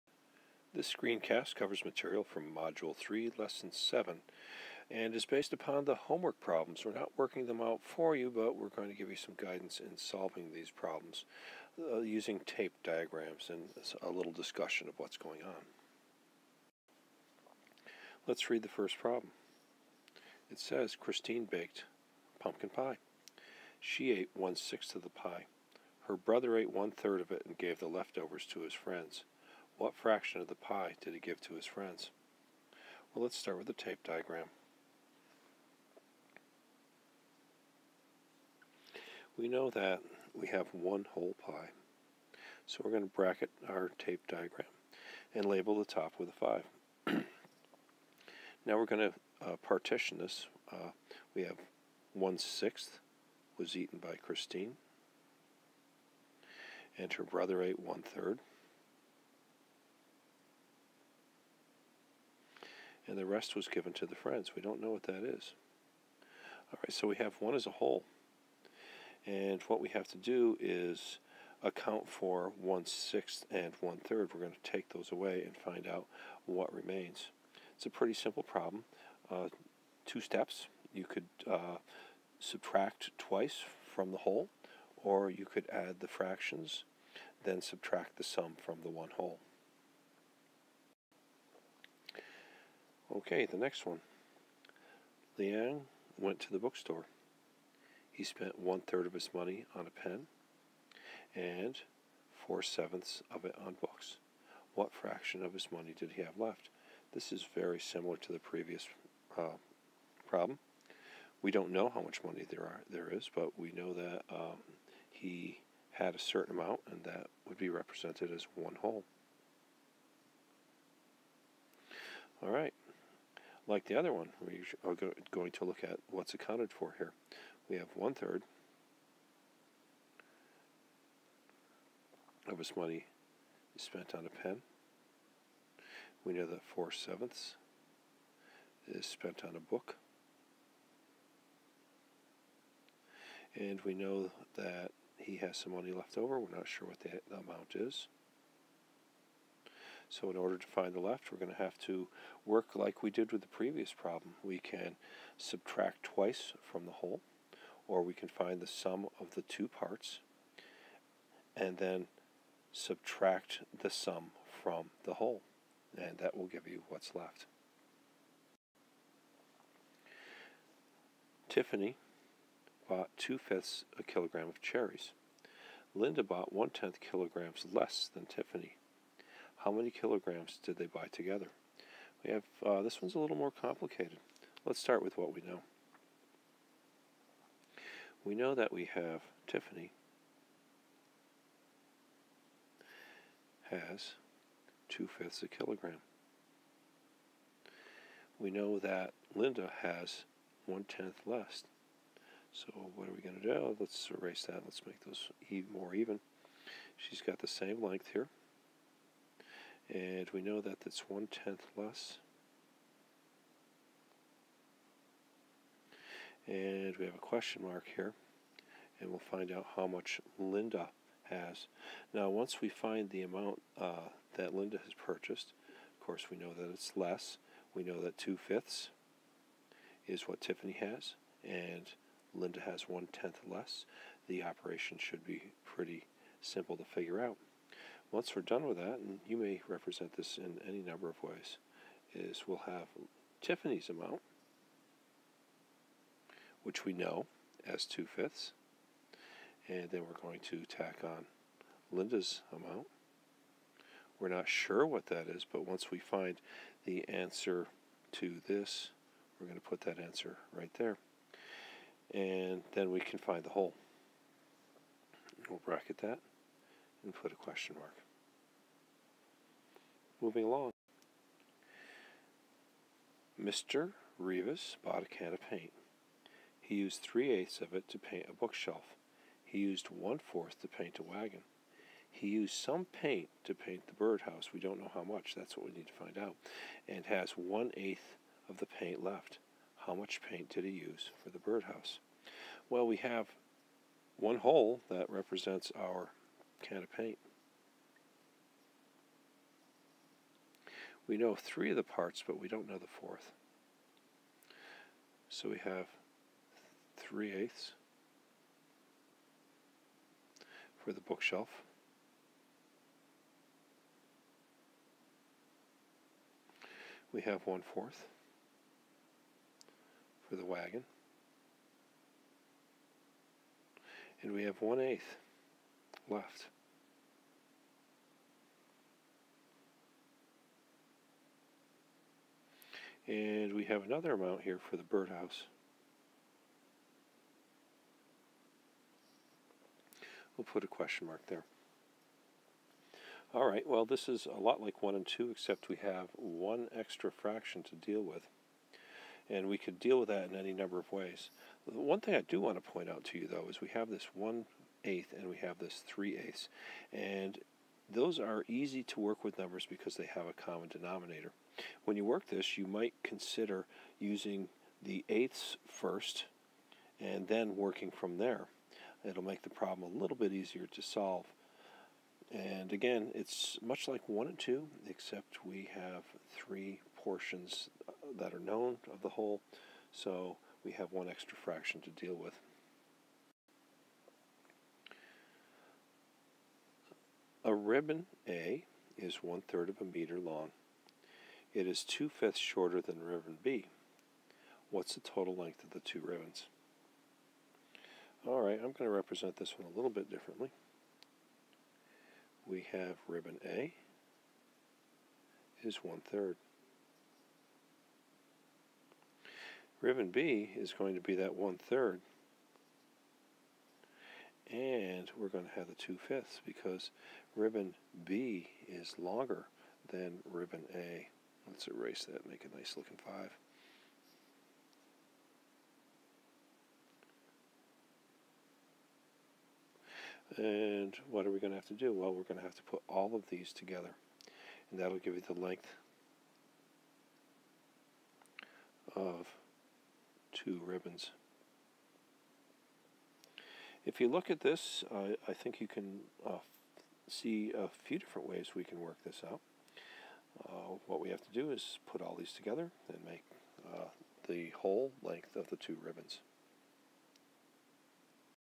Grade 5 EngageNY Eureka Math Module 3 Lesson 7 Homework Guidance
Solve two-step word problems with unlike fractions and mixed numbers.